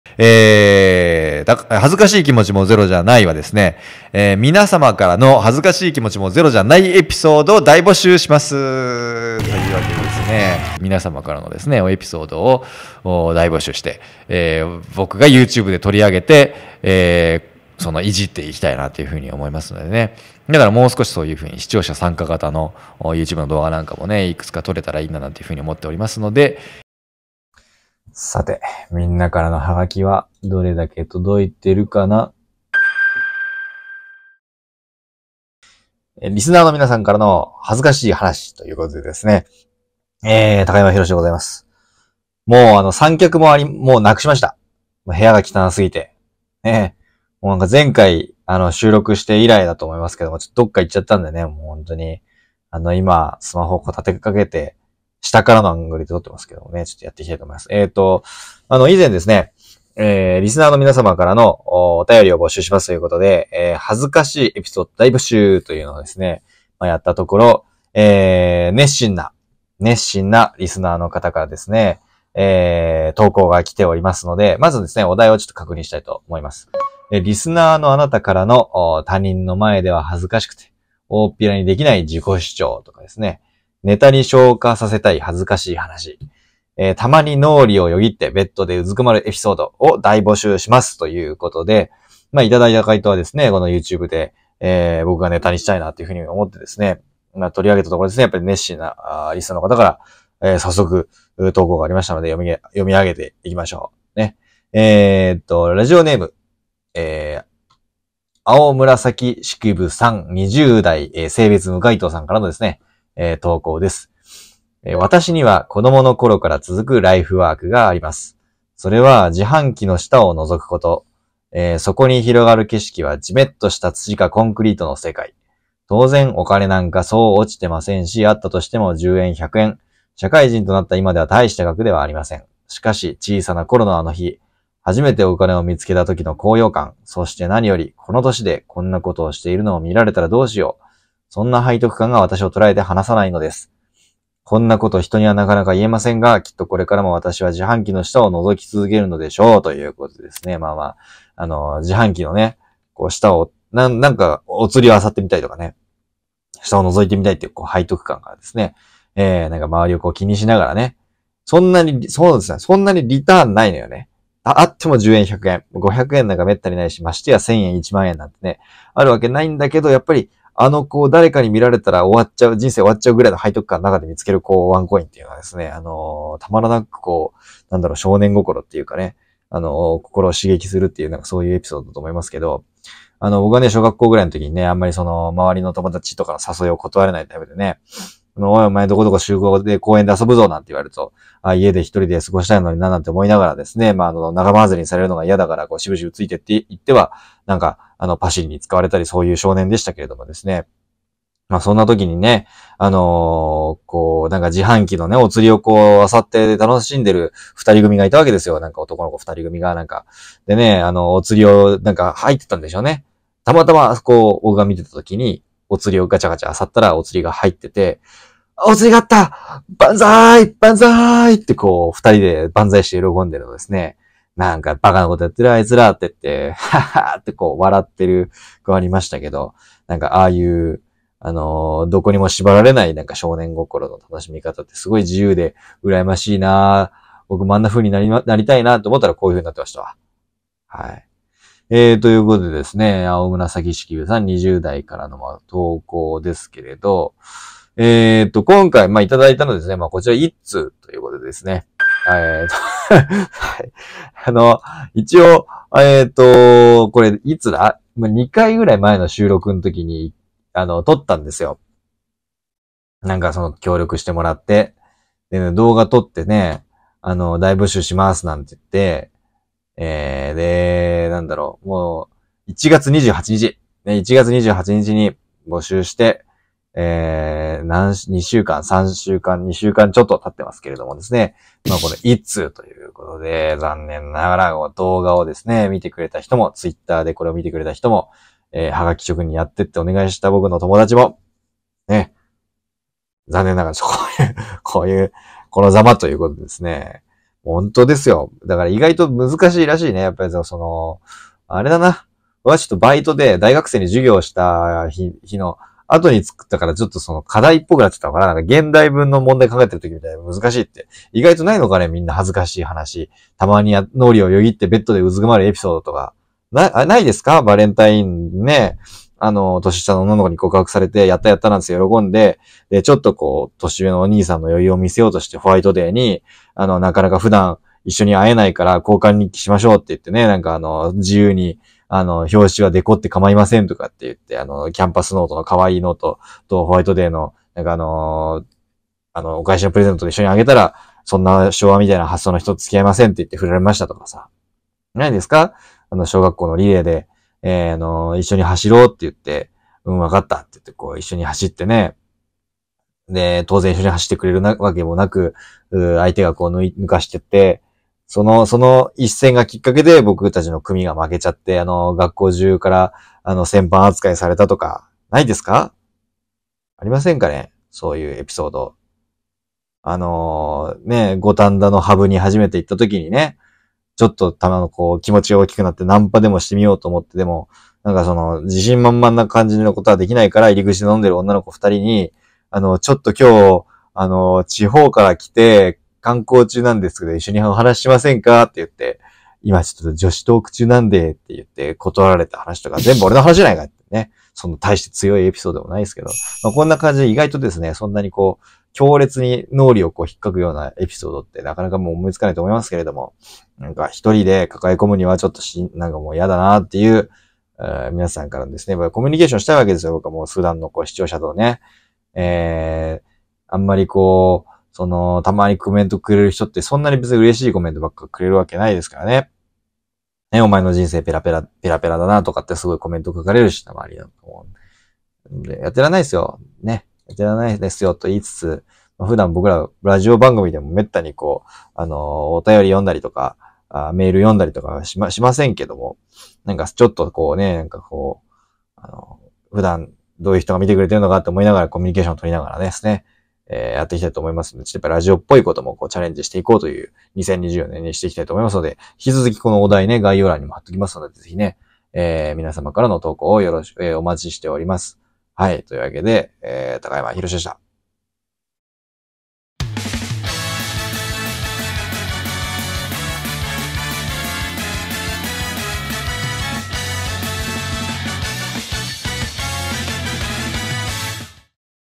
自販機の下を覗くアラサーの愉しみ…？｜ハガキ企画をやりたい！#1
「恥ずかしい気持ちもゼロじゃない」ではリスナーの皆さまからのエピソードをお待ちしております！！！

・他人の前では恥ずかしくて大っぴらにできない自己主張
・ネタに昇華させたい恥ずかしい話
・たまに脳裏をよぎってベッドでうずくまるエピソード

をこちらのURLからどしどし投稿くださいませ！

…という試みをやってみているものの、ネットの海で無視され続けている男の悲哀をご覧ください。